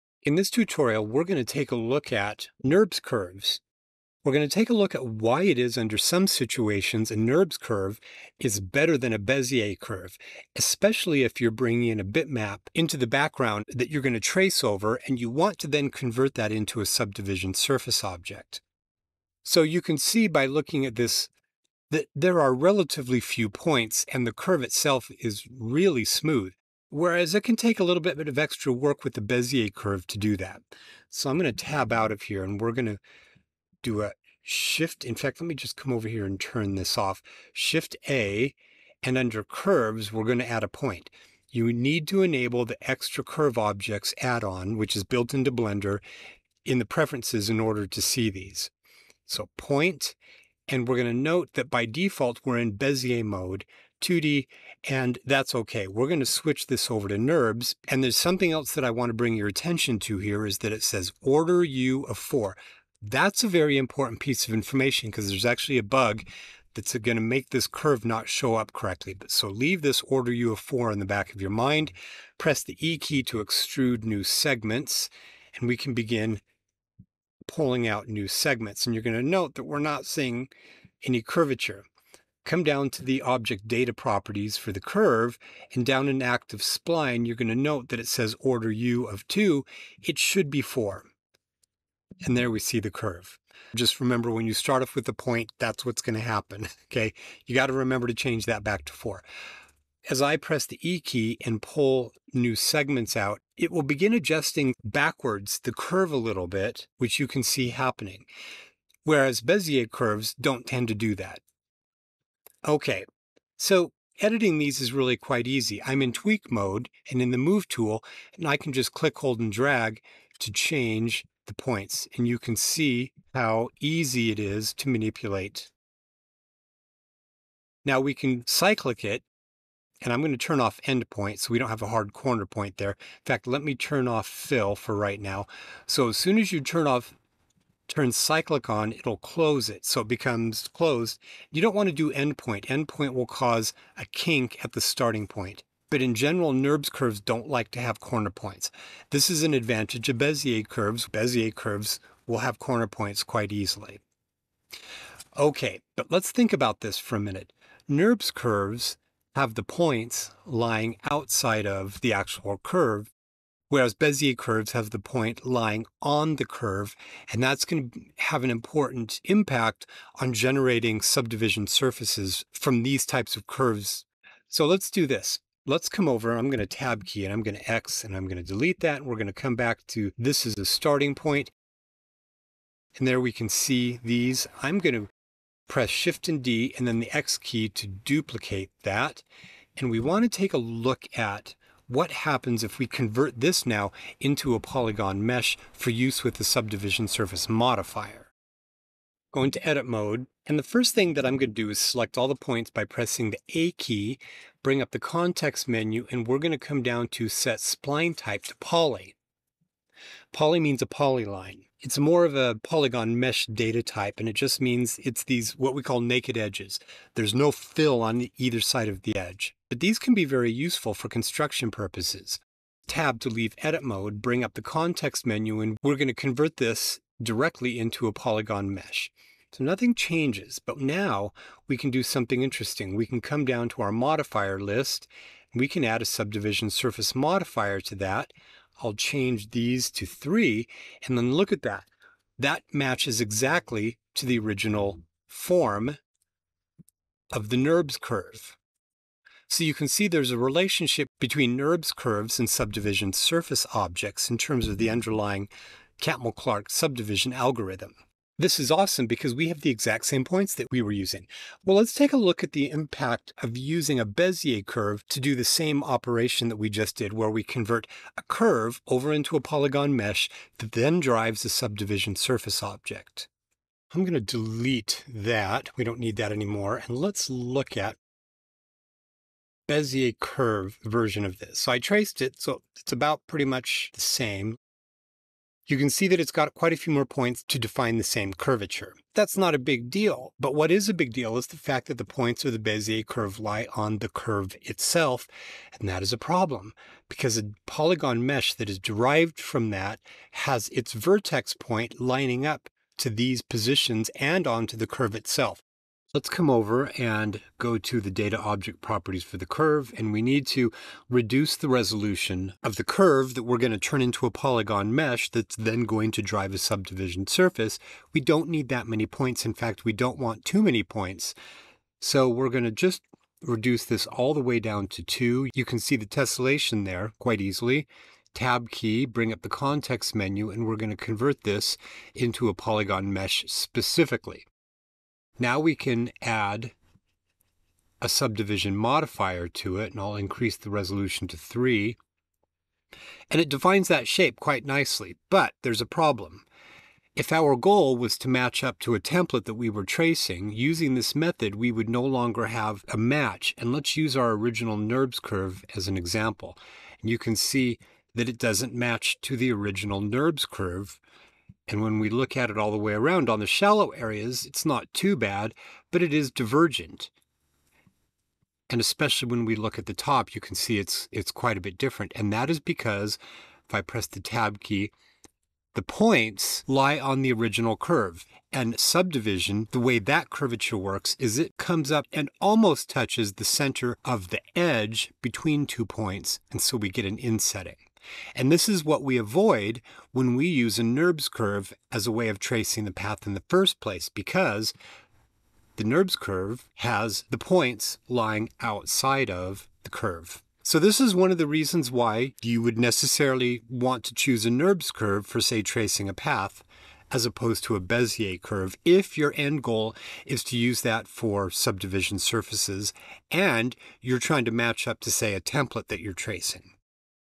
NURBS CURVES! Why the heck should I use them?.
Why would you want to use NURBS curves instead of bezier curves? Well, they have advantages when you want to generate a subdivision surface when using a curve as a starting point.